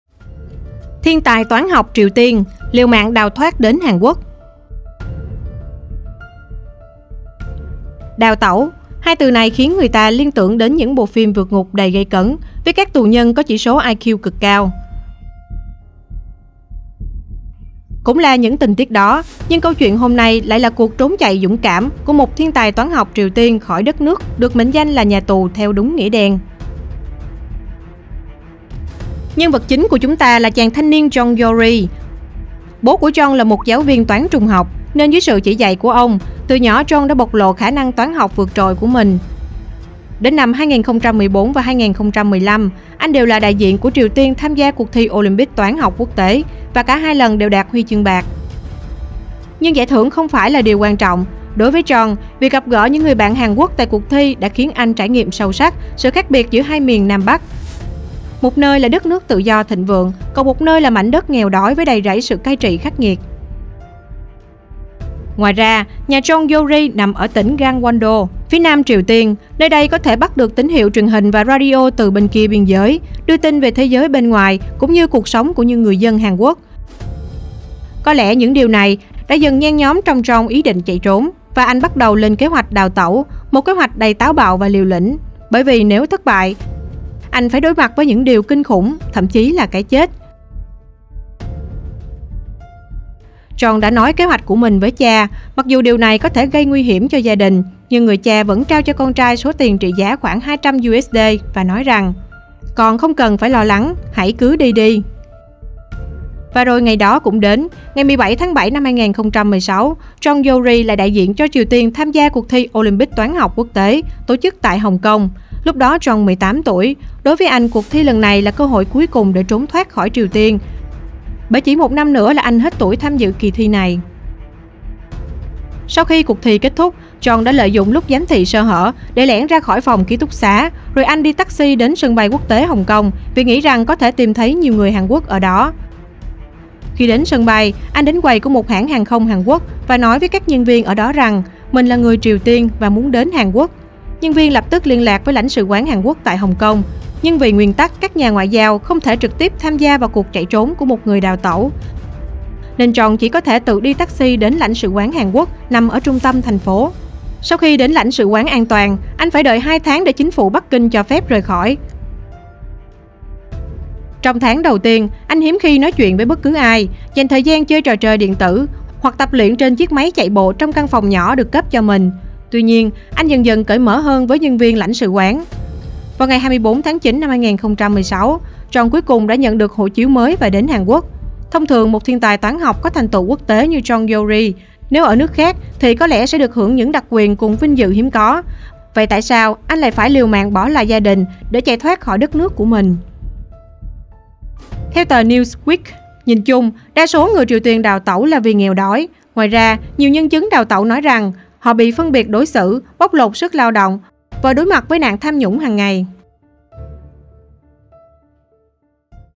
Thiên tài toán học Triều Tiên liều mạng đào thoát đến Hàn Quốc | Tinh Hoa TV
Đào tẩu -  2 từ này khiến người ta liên tưởng đến những bộ phim vượt ngục đầy gây cấn, với các tù nhân có chỉ số IQ cực cao.

Cũng là những tình tiết đó, nhưng câu chuyện hôm nay lại là cuộc trốn chạy dũng cảm của một thiên tài toán học Triều Tiên Jong Yol Ri khỏi đất nước được mệnh danh là “nhà tù” theo đúng nghĩa đen.